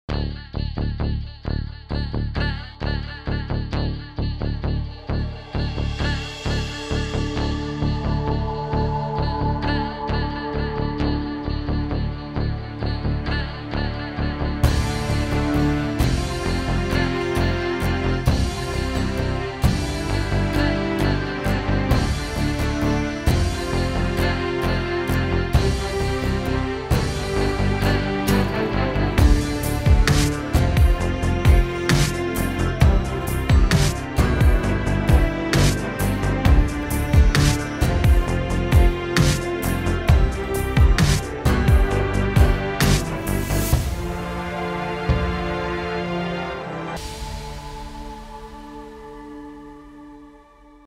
My First Cinematic Video, Danson Park, London
This is my first cinematic video, taken and edited by myself.